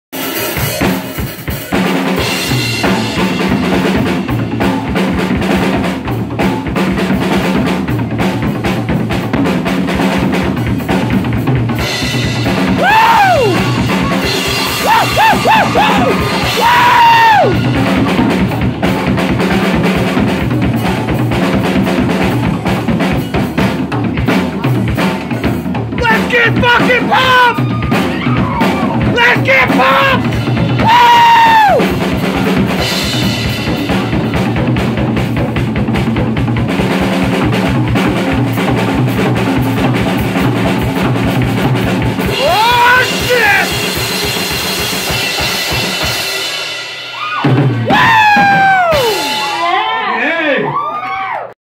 Drum Solo Tommy's Pub
listen for the dude going crazy in the back ground